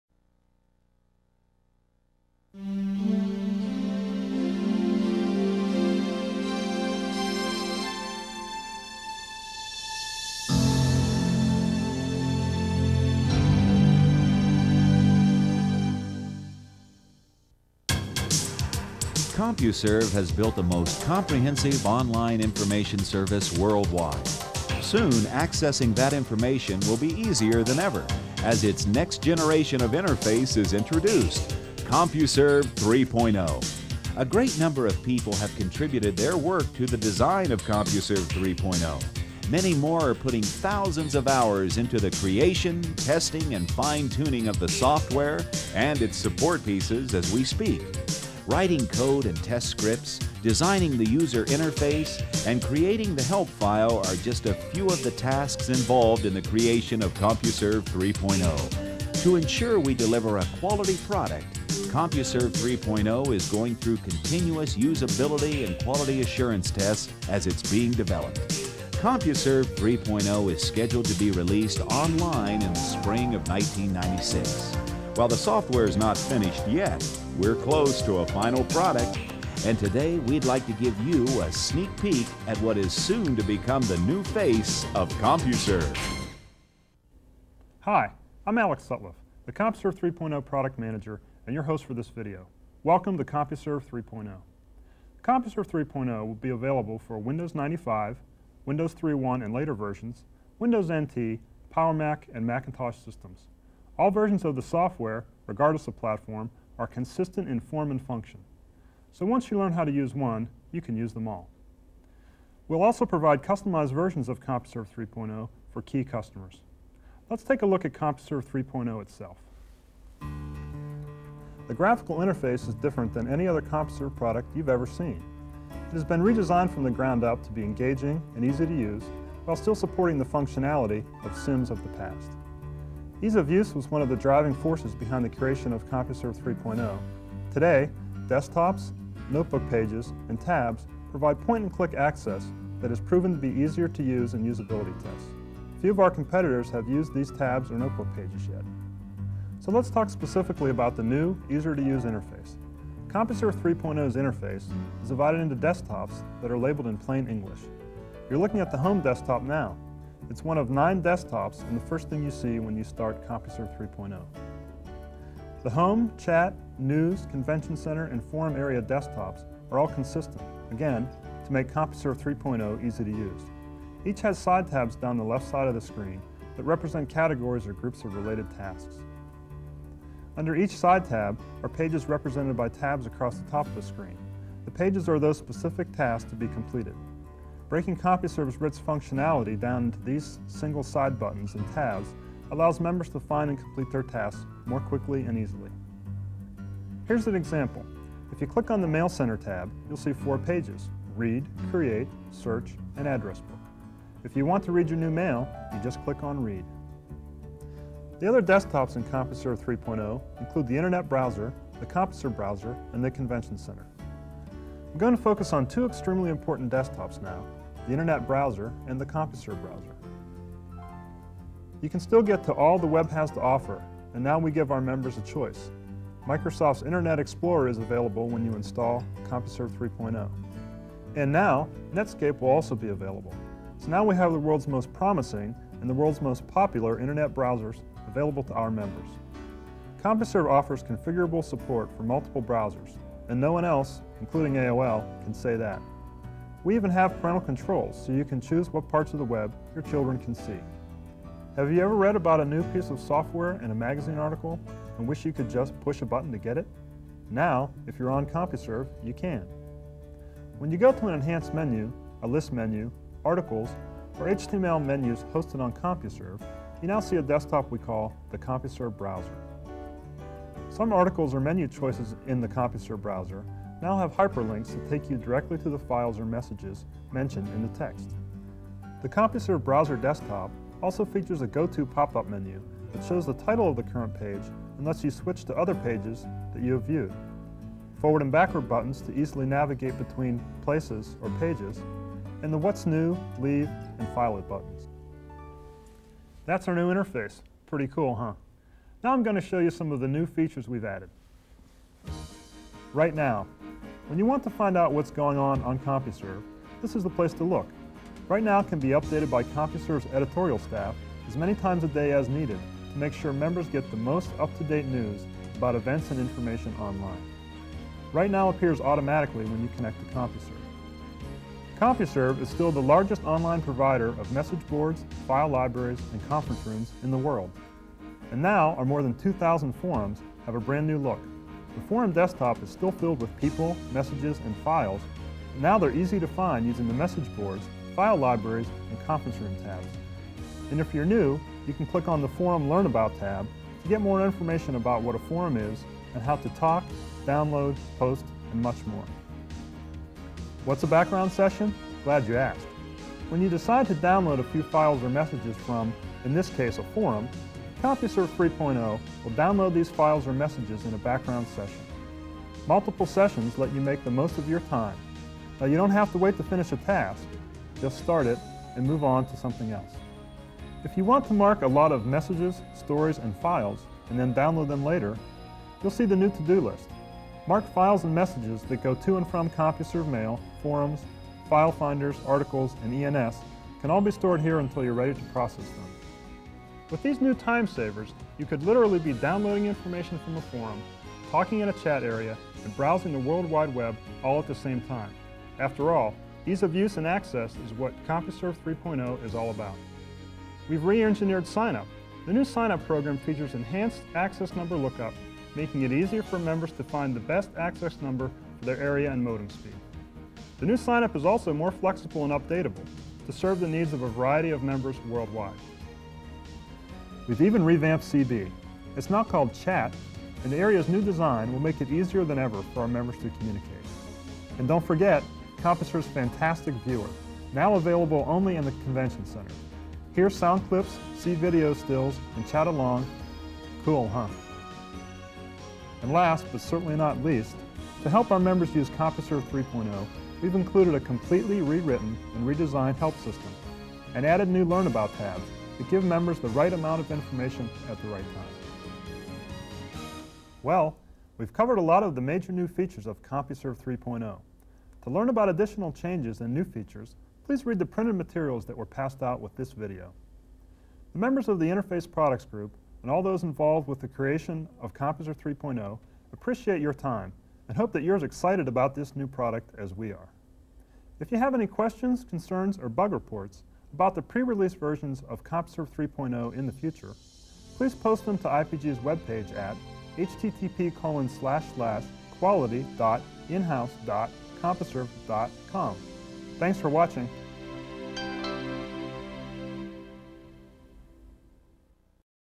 CompuServe 3.0 Sneak Peek
Internal video from the Documentation and Training Team in CompuServe's Interface Products Group (IPG) explaining new features in CompuServe 3.0 software (formerly called WinCIM) to pre-release testers. From ~1995.